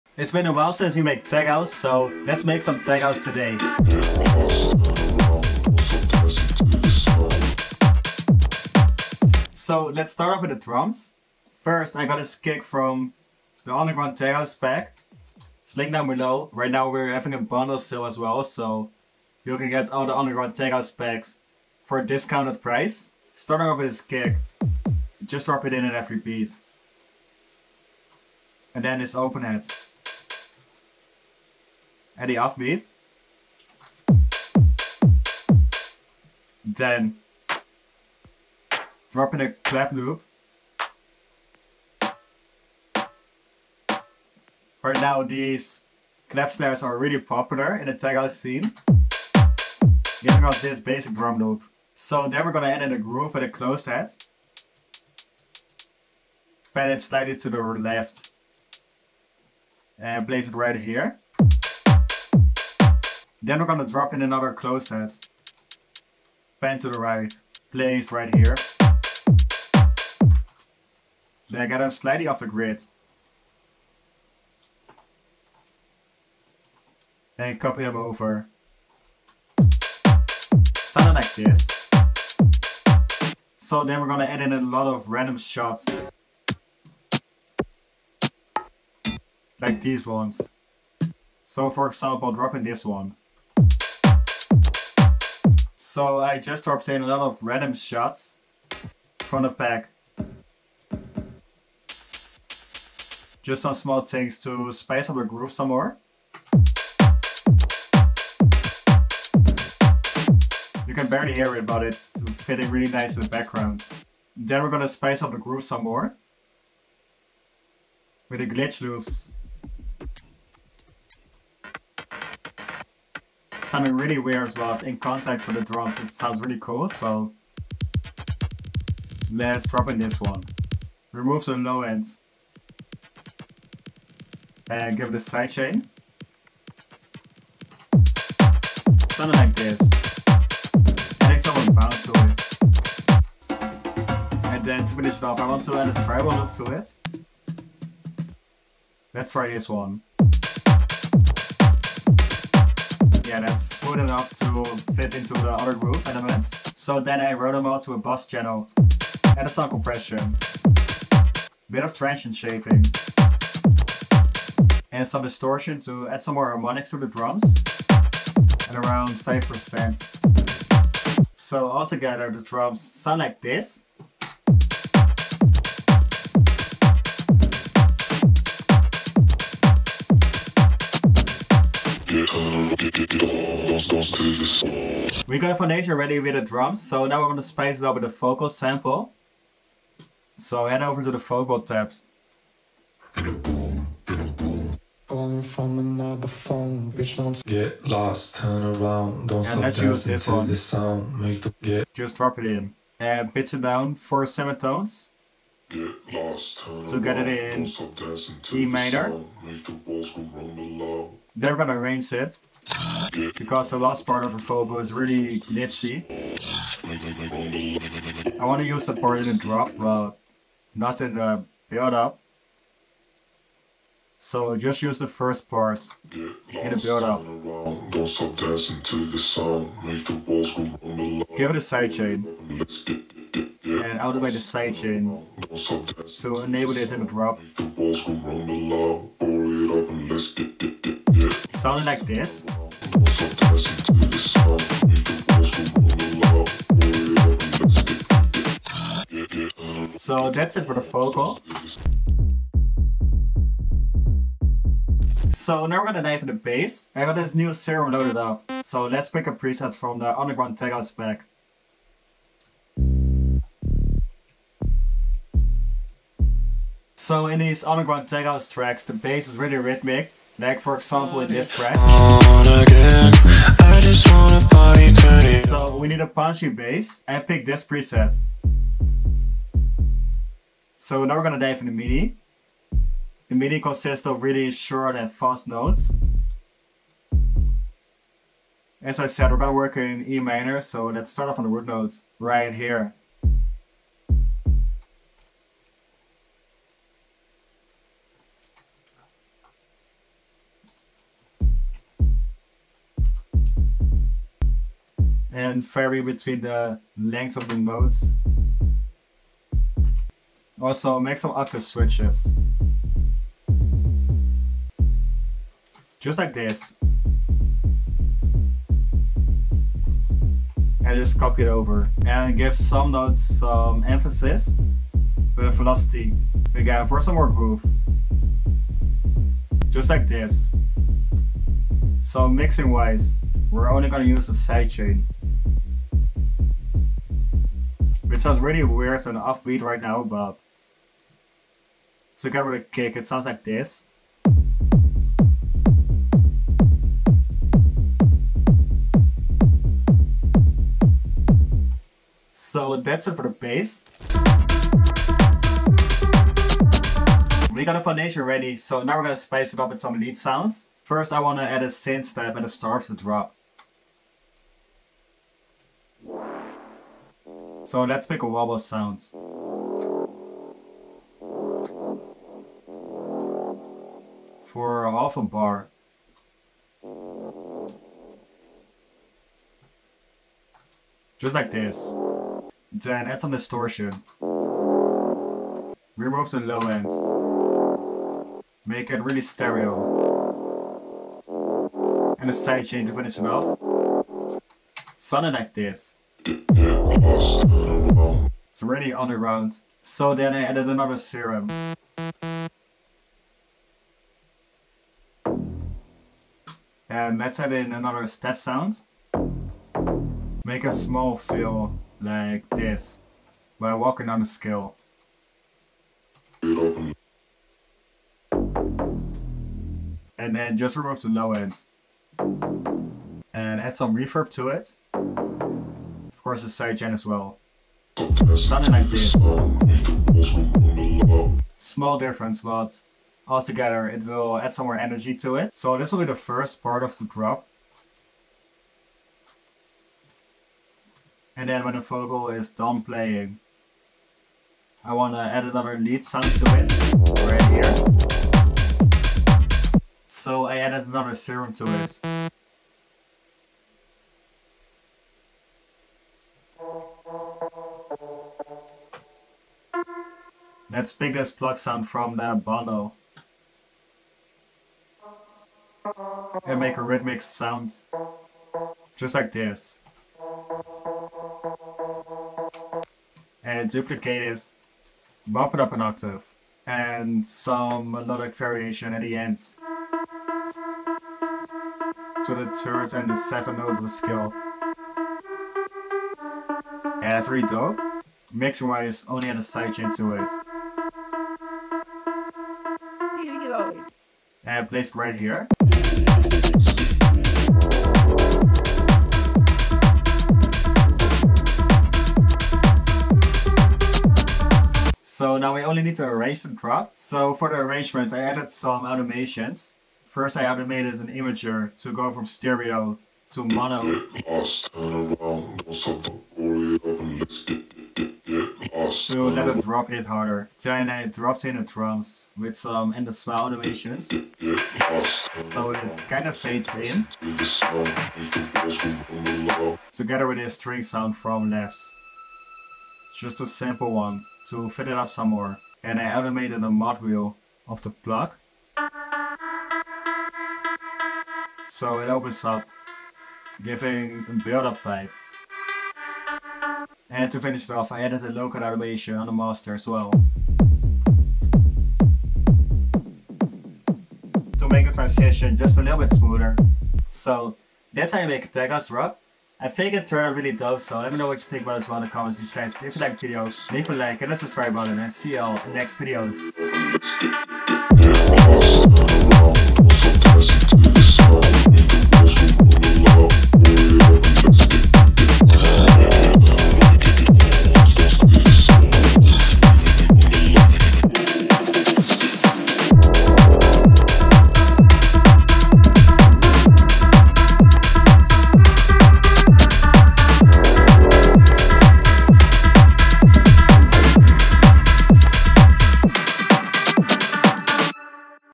POV: Producing Tech House [FL Studio Tutorial]
In this week's video we're gonna produce a tech house drop from scratch like tech house artists as Mau P and Fisher.

We'll be starting off with the tech house drums. First, we're gonna drop in the basic drums with the kick, clap & Hat. Then wer're adding in the tech house groove with the closed hats. Then, we're adding a glitch loop for that underground tech house feeling. To finish off the groove, we'll be adding in the percussion loop.

Then, it's time to add in the underground tech house bass. We'll be using a tech house bass preset from serum. After that, we're gonna draw in the tech house midi.

To fill it up some more, we're adding the tech house vocal sample as well. After the vocal, we're spicing up the drop a bit more with tech house wubbles, tech house stabs & a tech house lead in the second part of the drop.

So all together, I'll show you how to produce a tech house drop in FL Studio from scratch